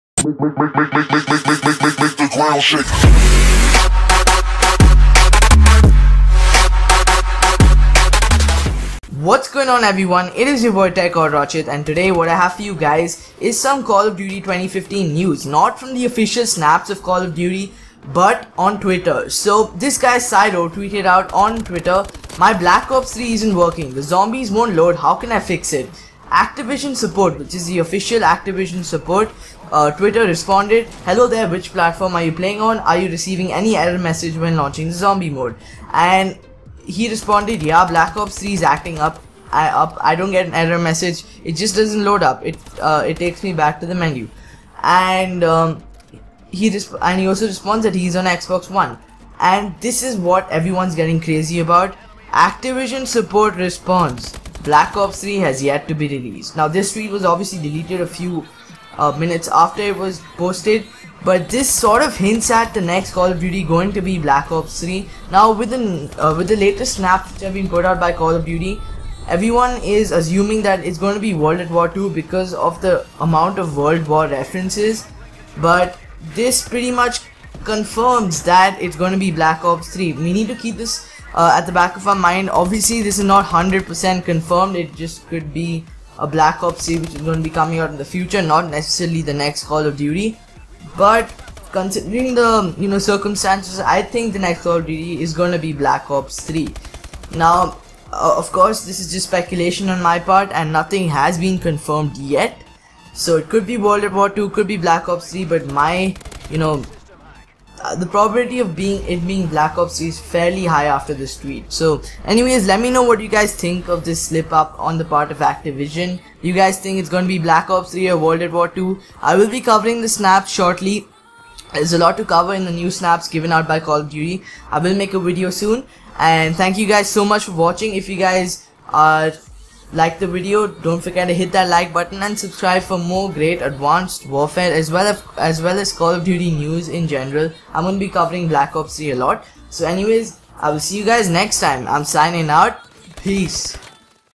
Activision officially Confirms Black Ops 3 as Call of Duty 2015 on Twitter!?
Do you think this confirms it? Pretty big news!
Pretty big slip up on the part of Activision!